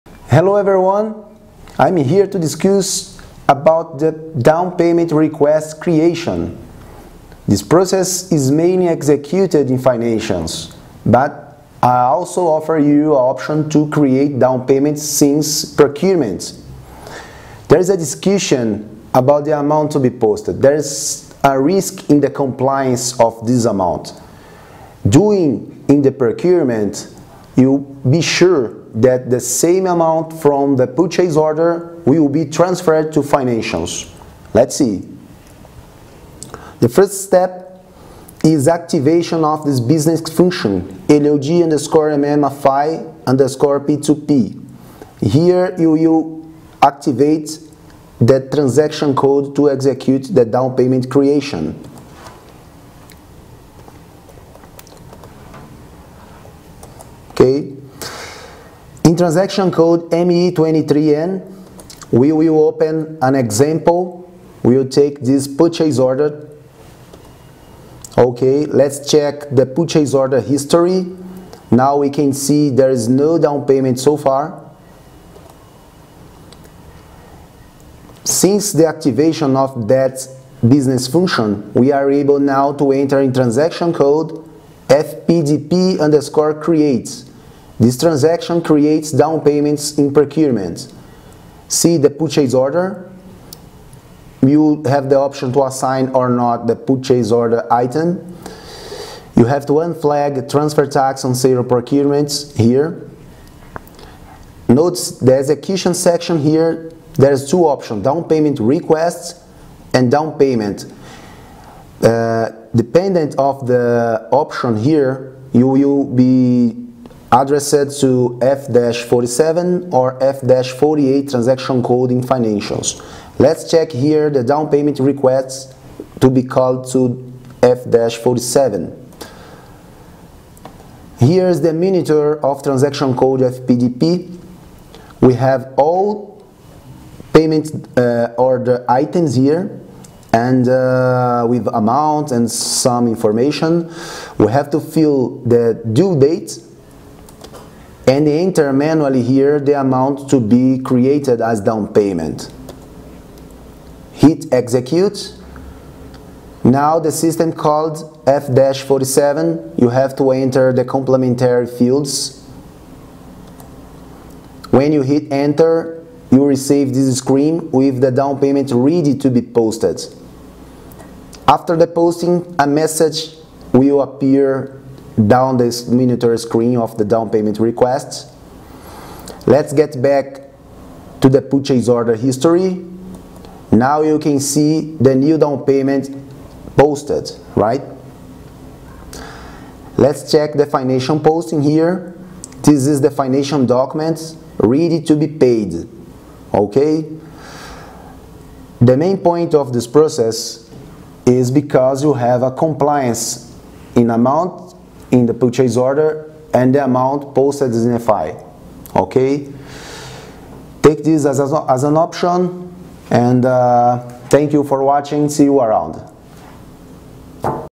Downpayments from Purchase Order
The Downpayment requests created in financials has no amount connection with purchase orders and there is a risk to have a different amount posted. Because this there is a way to create downpayments directly from Purchase Order through transaction FPDP_CREATE. Please check below walkthrough video.